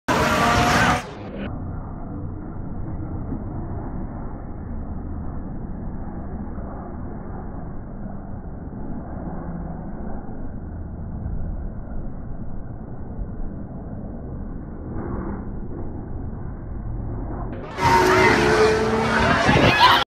Hamilton, Kvyat and Ricciardo
Hamilton, Kvyat and Ricciardo go by en route to turn 19 in the opening laps of the USGP.